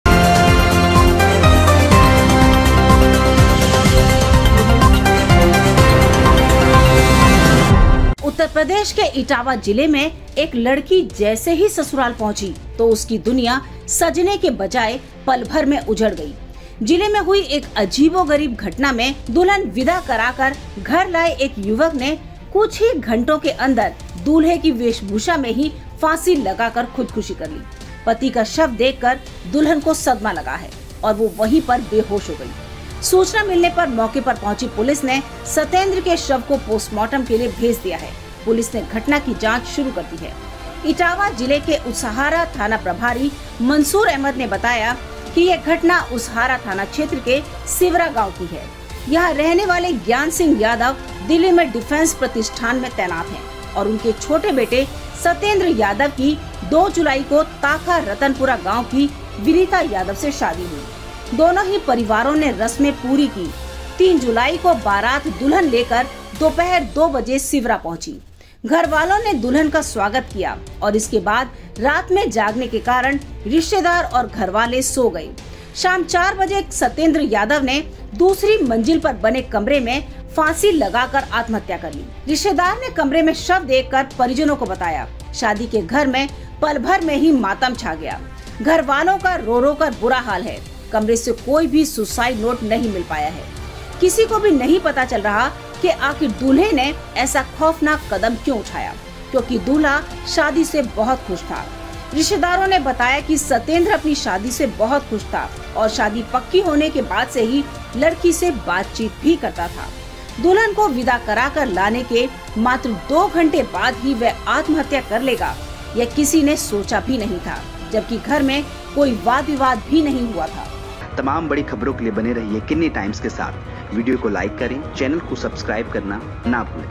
ETAWAH :घर वापसी के 2 घंटे बाद लटकती मिली दूल्हे की लाश, दुल्हन बेहोश; मंजर देख सिहर उठा पूरा परिवार
उत्तर प्रदेश के इटावा जिले में एक लड़की जैसे ही सुसराल पहुंची, तो उसकी दुनिया संजने की बजाय पलभर में उजड़ गई। जिले में हुई एक अजीबोगरीब घटना में दुल्हन विदा कराकर घर आए एक युवक ने कुछ ही घंटे के अंदर दूल्हे की वेशभूषा में फांसी लगाकर खुदकुशी कर ली। पति का शव देख दुल्हन को सदमा लग गया। वो वहीं बेहोश हो गई। सूचना मिलने पर मौके पर पहुंची पुलिस ने सतेन्द्र के शव को पोस्टमार्टम के लिए भेजा। पुलिस ने घटना की जांच शुरू कर दी है।